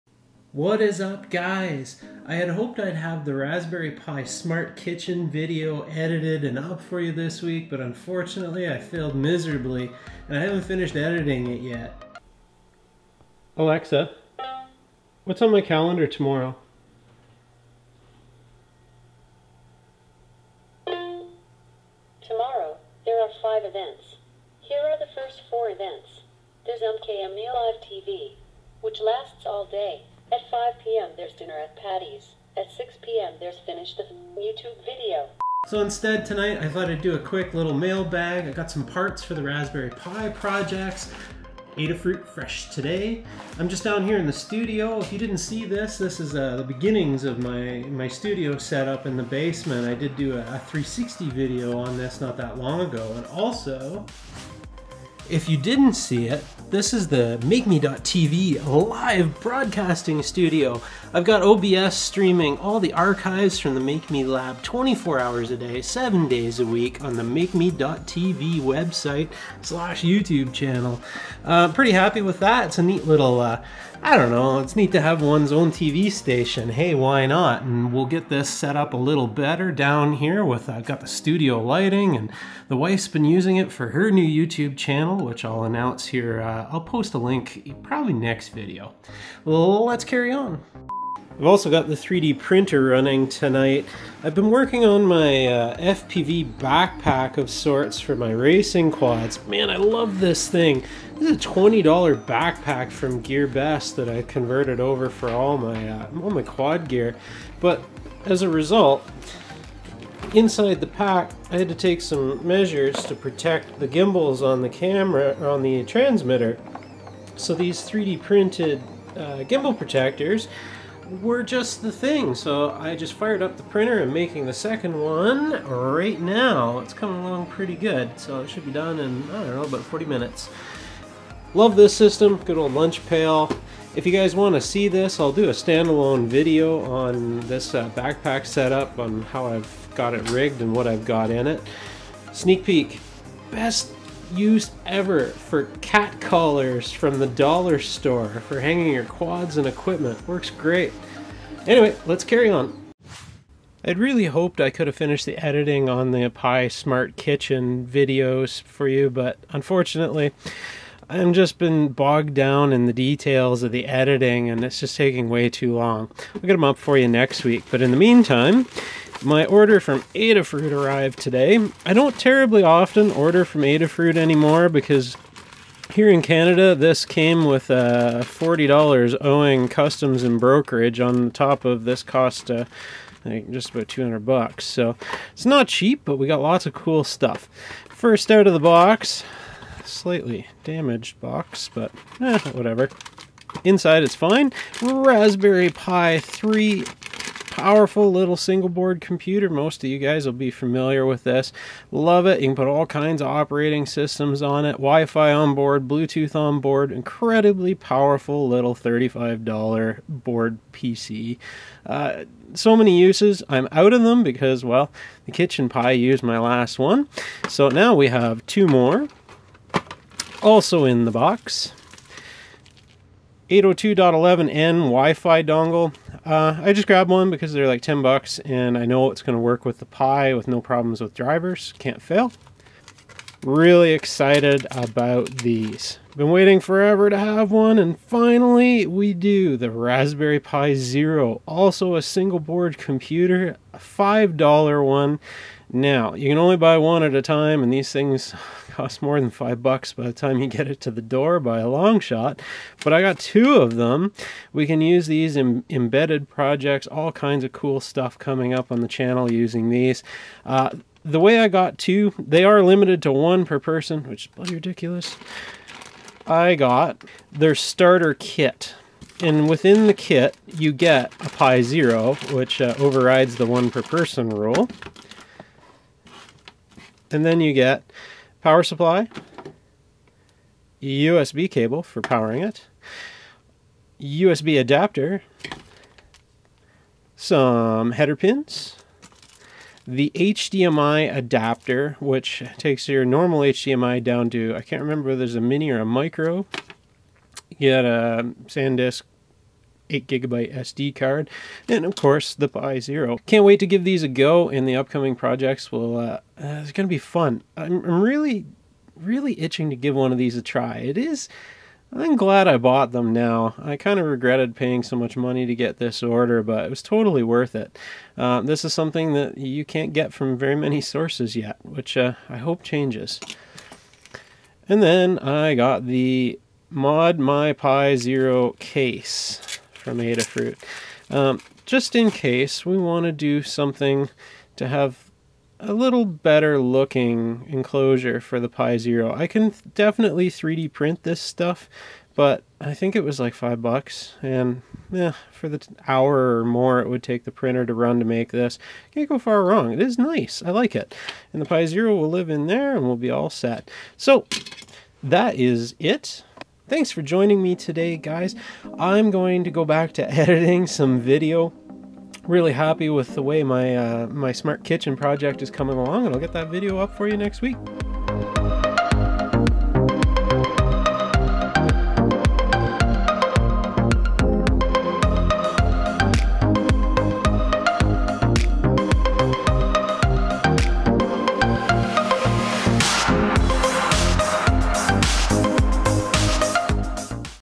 Making a Raspberry Pi Curse & How to NOT Make a Video...
First look at my new Raspberry Pi's and sneak peek at the smart kitchen project.

The Raspberry Pi Zeros will be great for ongoing projects and some IOT ideas I have.  Support Adafruit when you can for your electronics needs as they provide a tonne of great content free to all of us Makers and hobbyists.

Check back next week for the first tutorial on building your very own home automation / smart-home project with a Raspberry Pi and a simple LCD display.